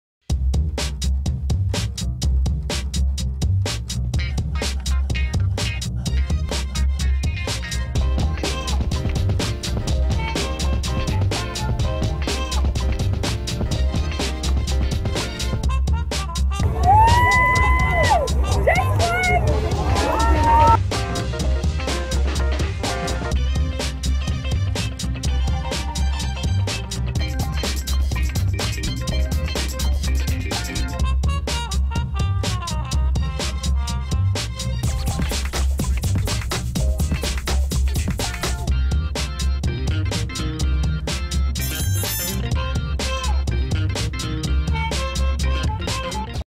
Klaxon Klick - Propulsore per carrozzine manuali
Il nostro propulsore per carrozzine manuali è maneggevole, scattante, veloce e sicuro. Un compagno insostituibile nella vita di ogni giorno, una gita fuori porta, un viaggio emozionante, per fare shopping, per vivere la natura, da soli o in compagnia.